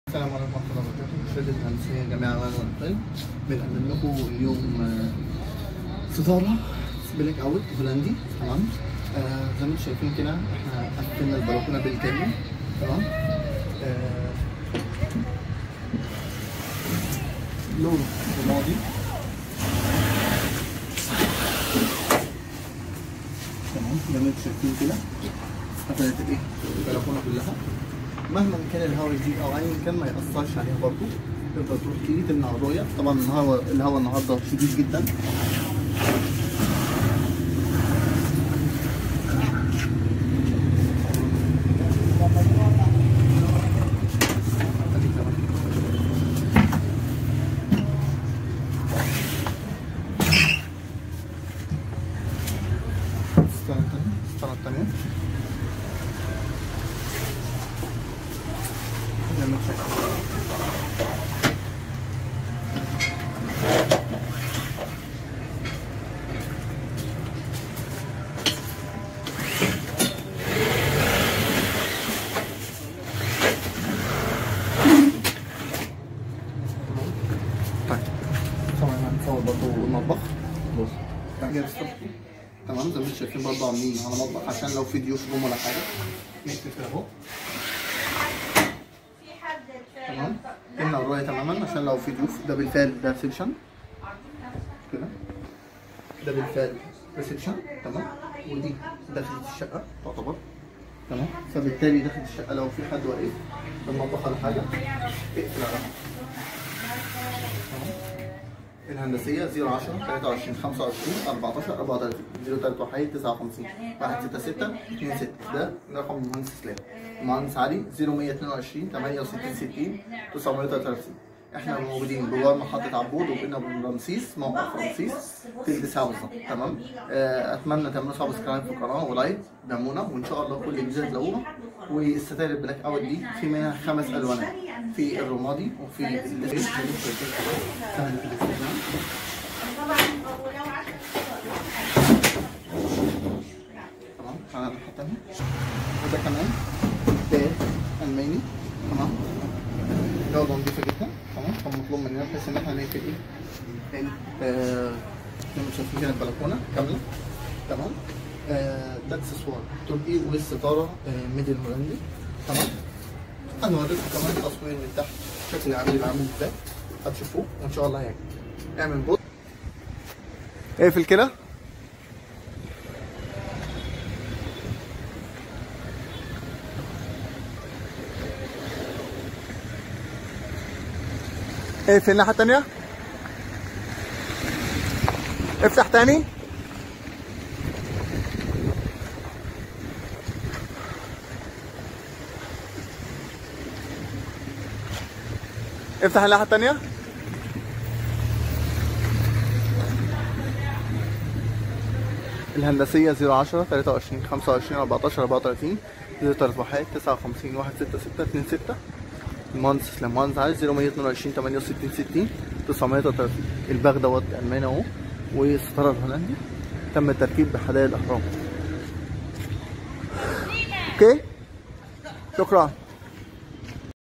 شوفوا جمال الستائر الهولندي للبلاكونه والمطابخ لسنة ٢٠٢١
ستارة بليسية بس صناعة هولندي
اللون رمادي
تم التركيب بحدائق الاهرام
مميزاتها :
١/ووتر بروف😍😍😍
  ٢/معتمه تماما لكي تحافظ علي خصوصية البيوت من تطفل الجيران 😍
٣/تحميك من الشمس والاتربه والمطر 😍
٤/تشغل حيز صغير وتقفل مساحات كبيره 😍
٥/سهله الحركه اللي بتساعد علي سهوله الاستخدام 😍
٦/يوجد لدينا ٨ الوان السنة دي بإذن الله وليس ٦ فقط كما ذكرت بالفيديو كمان 💪💪💪💪💪